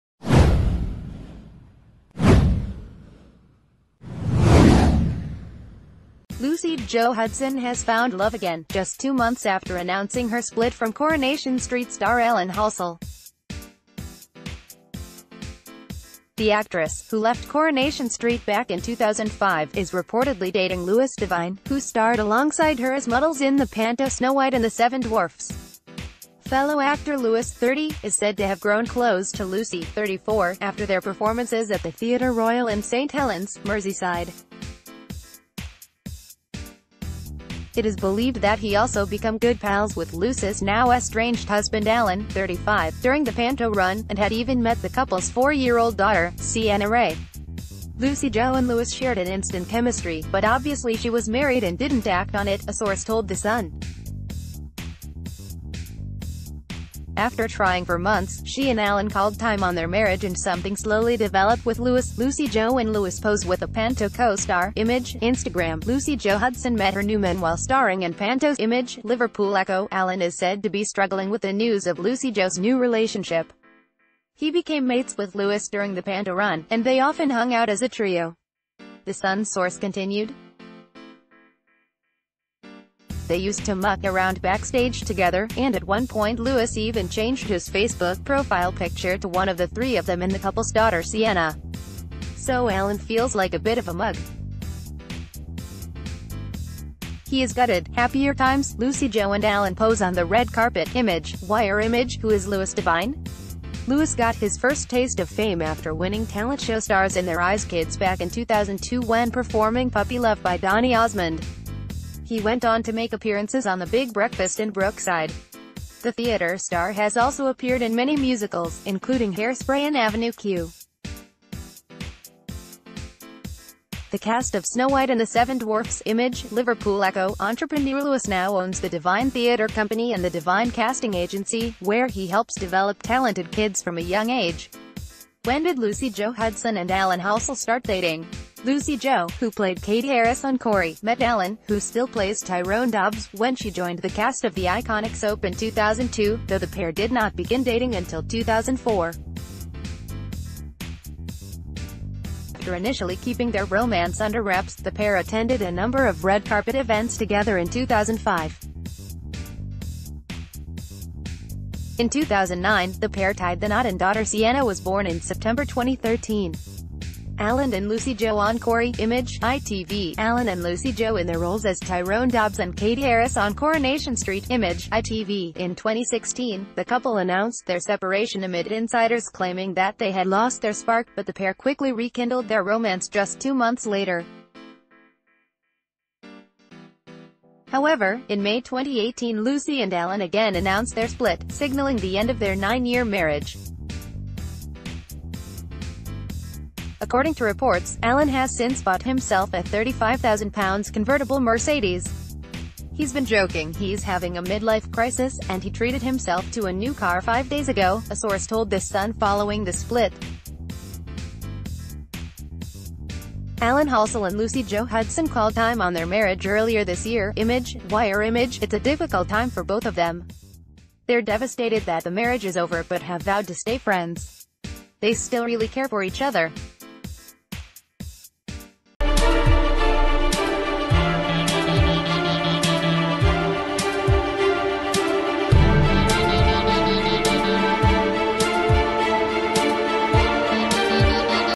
Lucy-Jo Hudson’s love life explained as Corrie star moves on after Alan Halsall
Lucy-Jo Hudson has found love again, just two months after announcing her split from Coronation Street star Alan Halsall. The actress, who left Coronation Street back in 2005, is reportedly dating Lewis Devine, who starred alongside her as Muddles in the panto Snow White and the Seven Dwarfs. Fellow actor Lewis, 30, is said to have grown close to Lucy, 34, after their performances at the Theatre Royal in St Helens, Merseyside. It is believed that he also become good pals with Lucy’s now-estrange...